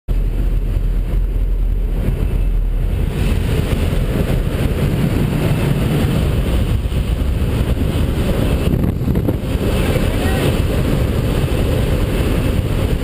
How to accidentally pull your cutaway handle in the airplane door frame skydiving. Friday Freakout
Check your handles